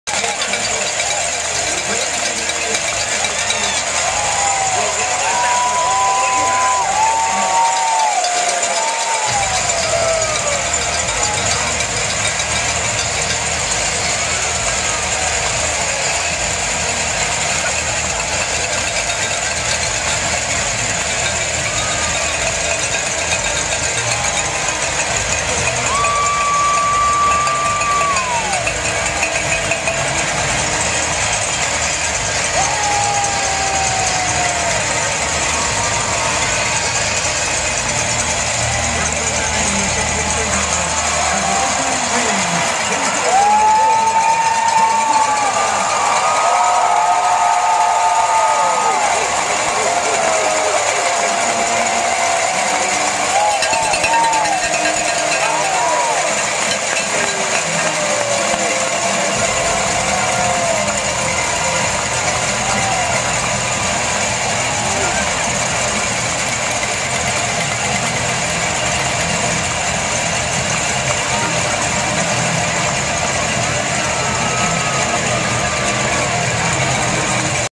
2011 Music City Bowl - MSU entrance
Mississippi State takes the field for the 2011 Music City Bowl at LP Field in Nashville, Tennessee on December 30, 2011. Mississippi State won the game, 23-17.